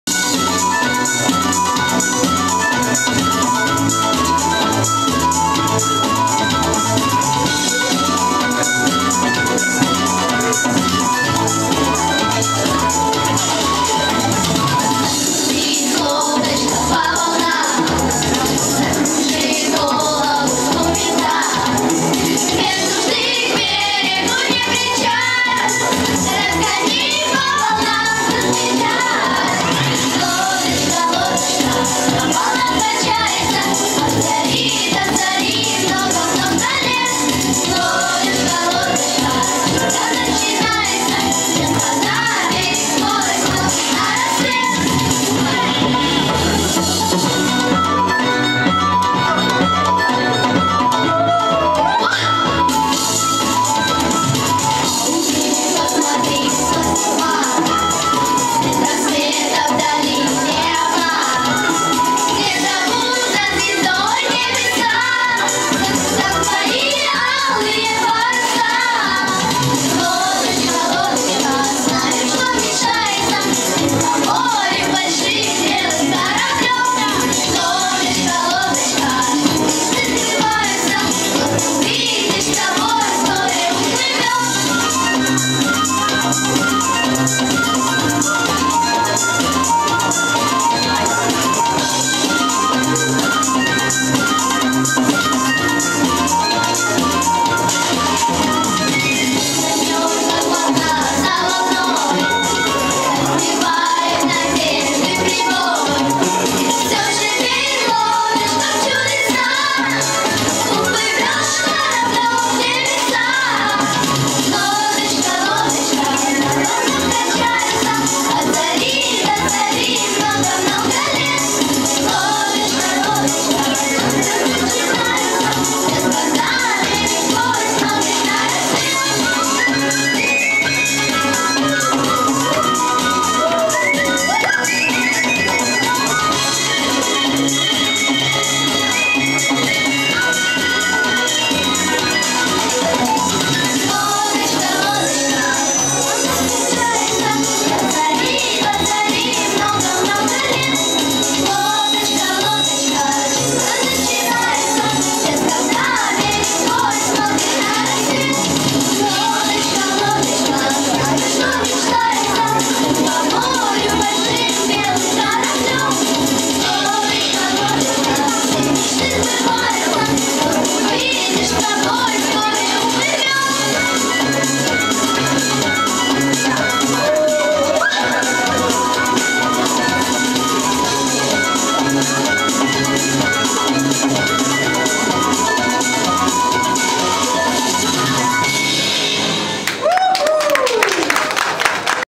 Веселая масленица в ТЦ JAMBO Кишинев. Коллектив хора "Лира"
Ярмарка подарков и сувениров от рукодельниц и народных мастеров. Коллектив "Лира"- под руководством Болотовой Е. В.(учитель музыкального воспитания и дирижер старшего хора «Лира» теоретического лицея «Михай Греку» г. Кишинев.)